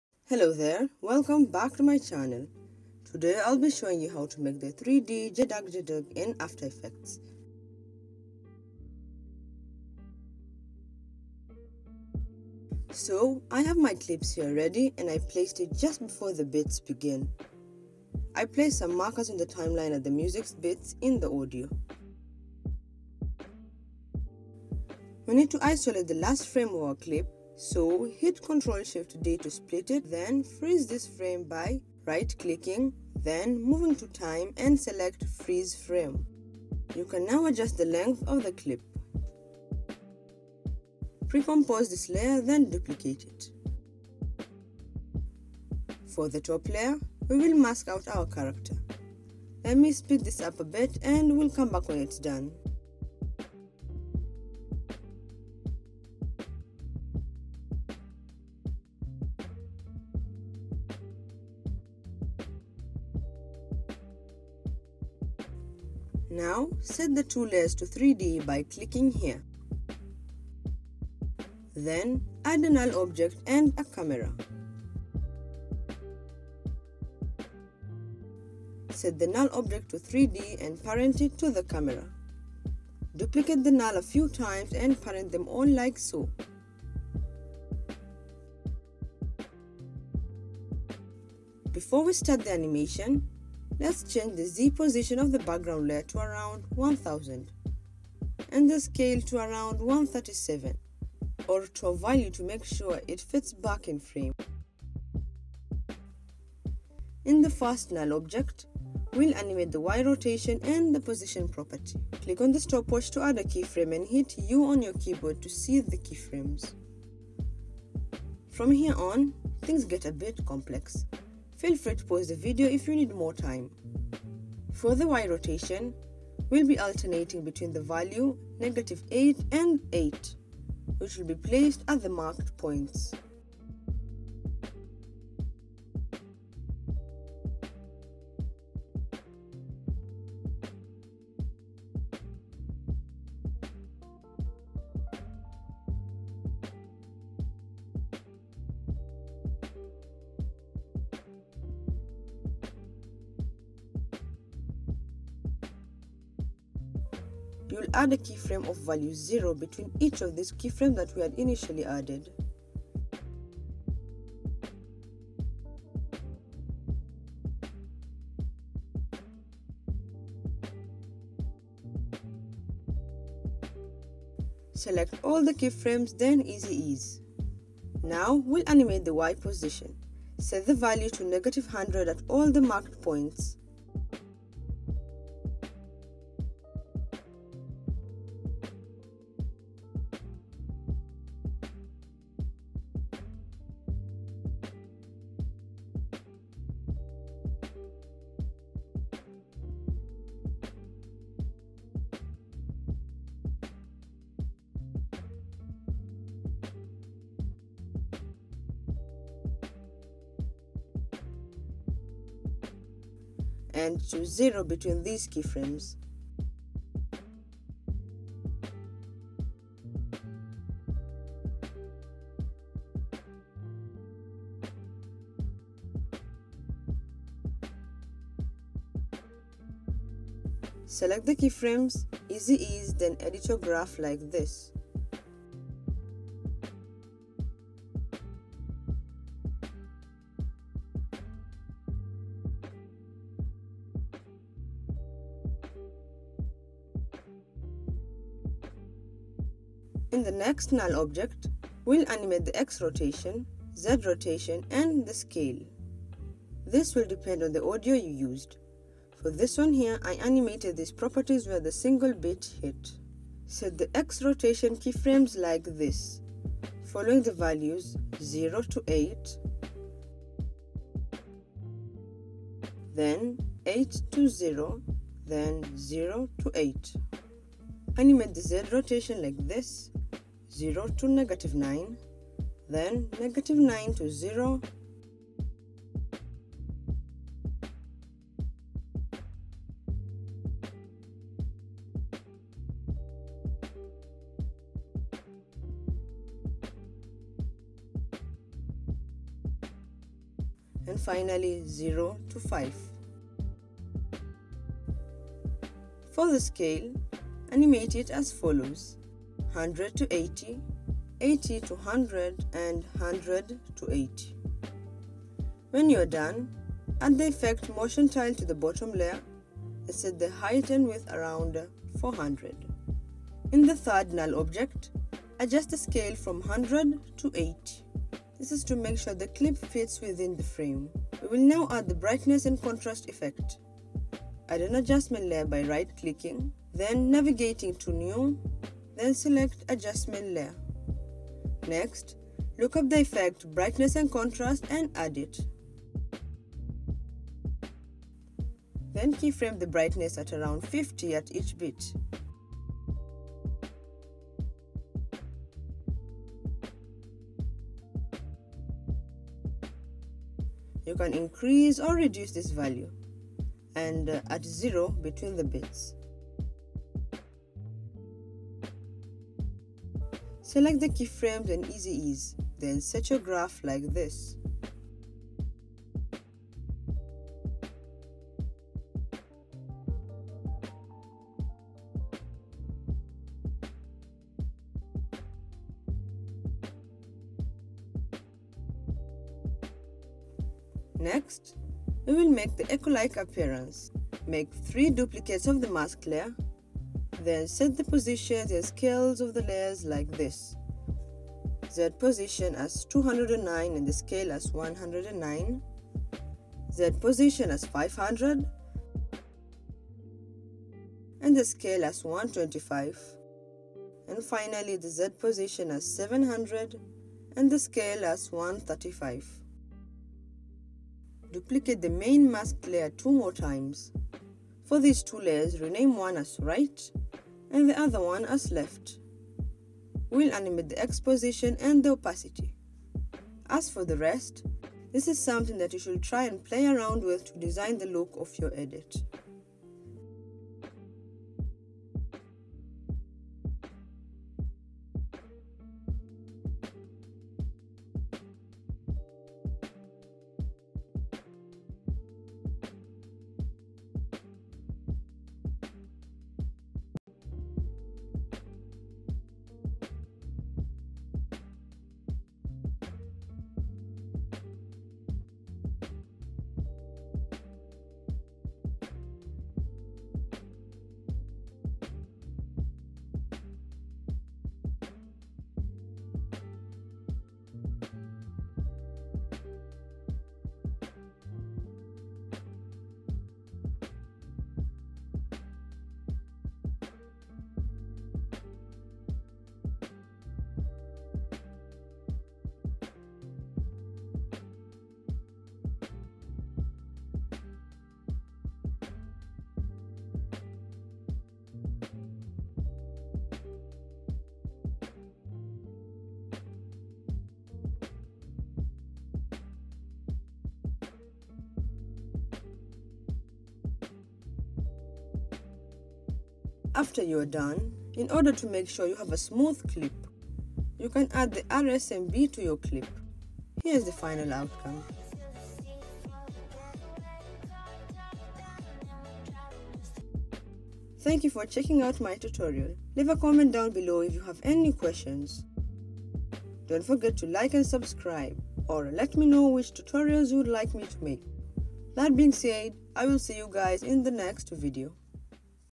3D JEDAG JEDUG like Shoko : After Effects Tutorial
How to make the  3D Jedag Jedag in After Effects

So, I have my clip here ready and I placed it just before the beats begin. I have placed some markers on the timeline at  the music's beats in the audio.

We need to isolate the last frame of our clip, so hit ctrl shift D to split it then freeze this frame by, right clicking, then moving to time then select freeze frame. You can now adjust the length of the clip.

Precompose this layer then duplicate it. For the top layer, we will mask out our character. Let me speed this up a bit and we'll come back when it's done.

Now, set the 2 layers to 3d by clicking here. Then add a null object and a camera. Set the null object to 3d and parent it to the camera. Duplicate the null a few times and parent them all like so.

Before we start the animation. Let's change the z position of the background layer to around 1000, and the scale to around 137 or to a value to make sure. It fits back in frame. This is what brings about the 3d look when we rotate it.

In the first null object, we will animate the y- rotation and the position property.

Click on the stopwatch to add a keyframe and hit 'u' on your keyboard to see the  keyframes

From here on things get a bit complex, feel free to pause the video if you need more time.

For the y rotation. We will be alternating the value between '-8' and '8' which will be placed at the marked points. When you are done. You will add a keyframe of value '0' between each of the keyframes we had added. Select all the keyframes then easy ease them.

Now we'll animate the y position. Set the value to '-100' at all the marked points. And to '0' between these keyframes. Select the keyframes, easy ease then edit your graph like this.

In the next null object, we will animate the x rotation, z rotation and the scale.

This will depend on the audio you used. For this one here, I animated these properties where the single beat hit.

Set the keyframes like this. Following the values, 0 TO 8. Then 8 to 0. Then 0 to 8.

Animate z rotation Setting the keyframes from  0 to -9. Then - 9 to 0 then 0 to 5

For the scale, animate it as follows.

100 to 80, 80 to 100 them 100 to 80.

When you are done. Add the effect motion tile to the bottom layer. And set the height and width around 400.

In the 3rd null object, adjust the scale from 100 to 80.

We will now add the brightness and contrast effect. Add an adjustment layer by right clicking, then navigating to new then select Adjustment layer

Next, look up the effect brightness and contrast and add it.

Then Keyframe the brightness at around 50 at each beat. You can increase or reduce this value. And 0 between the beats. Select the keyframes and easy ease, Then set your graph like this.

Next, we will make the echo-like appearance. Make 3 duplicates of the masked layer Then Set the positions and scales of the  layers like this.

Z position as 209 and scale as 109

Z position as 500 and scale as 125

Z position as 700 and scale as 135

Then duplicate the main masked layer 2 more times. For these 2, rename one as  'right' and the other 'left'. We will animate the x position and the opacity. Follow what I do.

As for The rest, this is something that you should try and play around with to design the look of your edit.